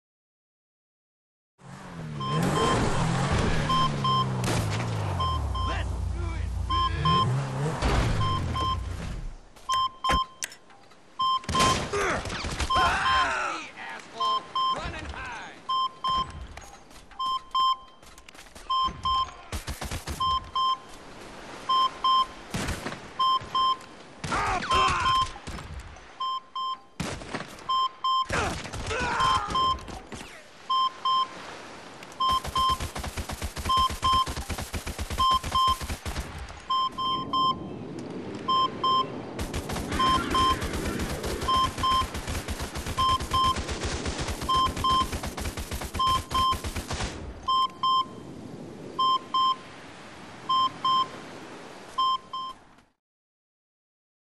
Crate "Dropped" But NO Crate! wtf lol...
Went to a crate drop, first one there, killed the pedestrian guys that were there and then noticed that there IS NO CRATE AT ALL ANYWHERE! I checked around it, up and down the hills, on the beach, everywhere... but nothing lol.